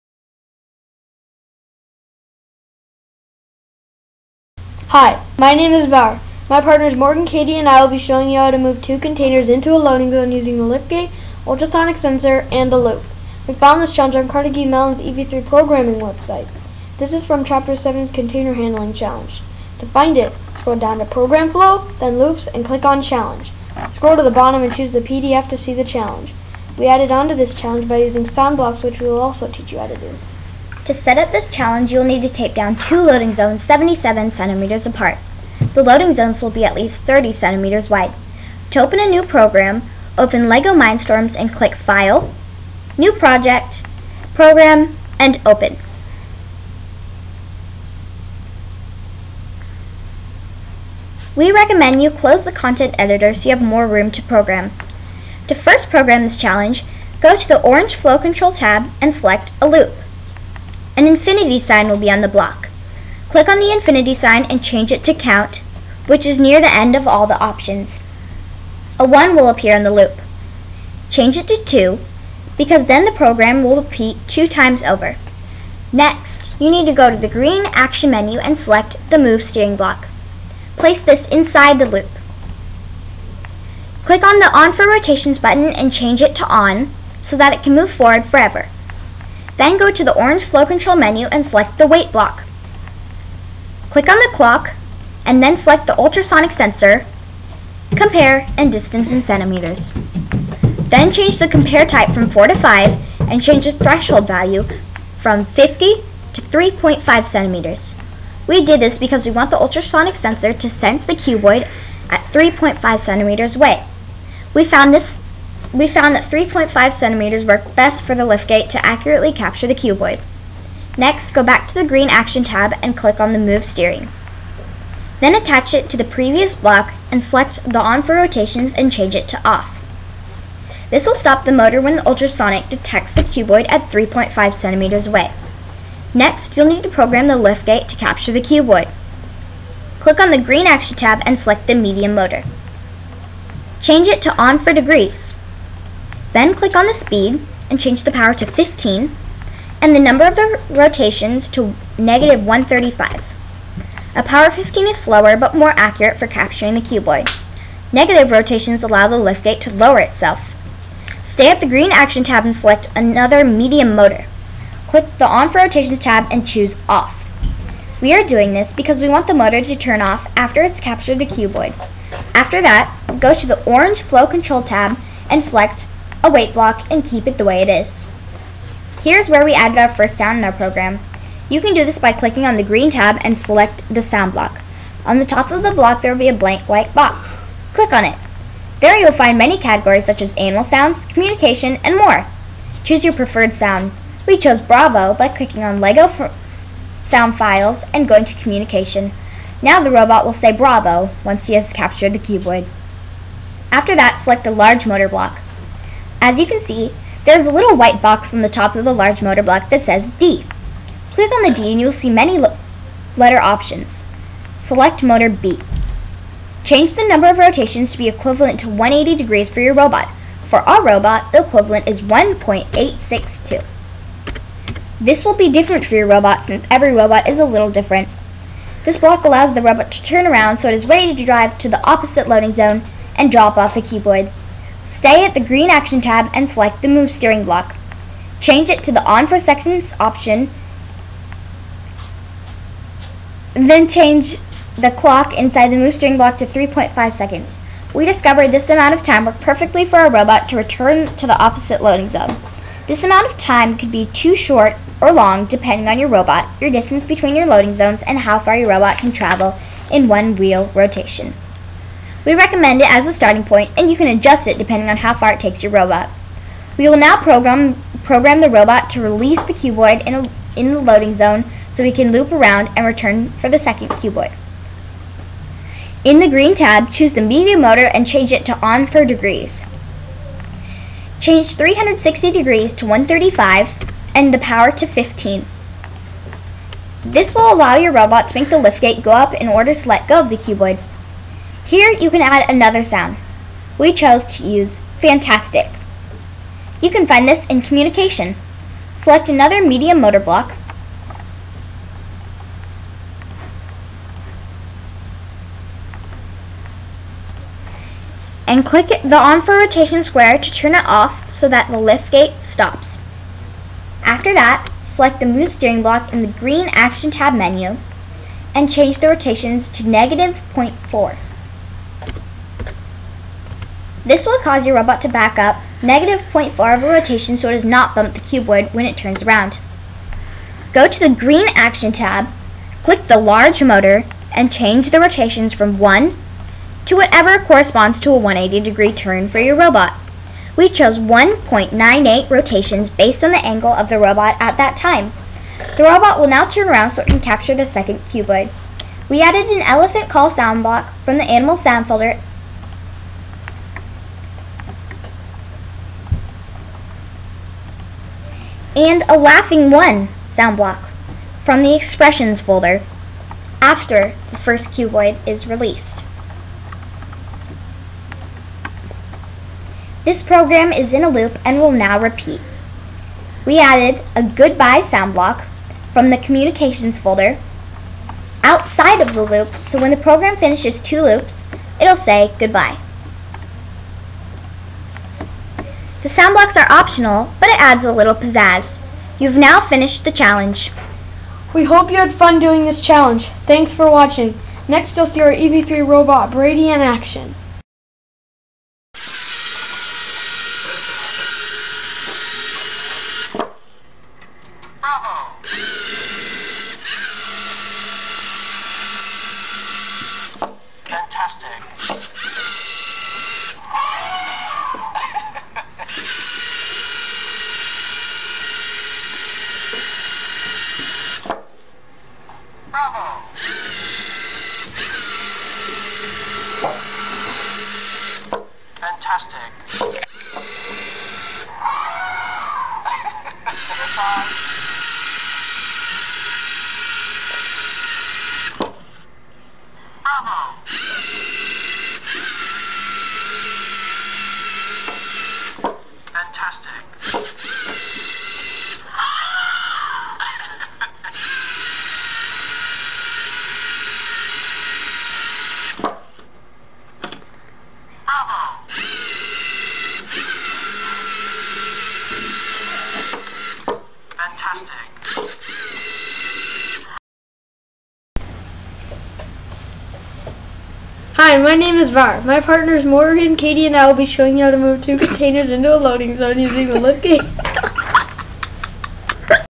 How to program your EV3 robot to make sounds and use the lift gate
Katie, Morgan, and Bauer will teach you how to move your EV3 lift gate in a loop while playing sounds.